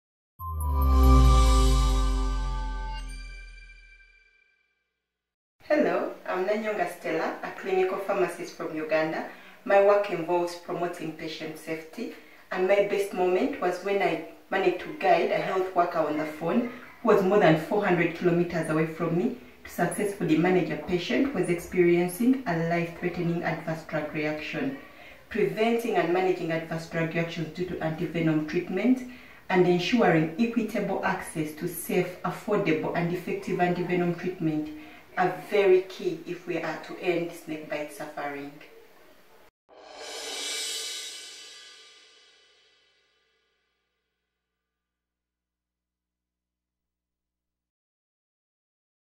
Stella Nanyonga - 2020 Women Champions of Snakebite
Stella is a clinical pharmacist and epidemiologist in Kampala Uganda.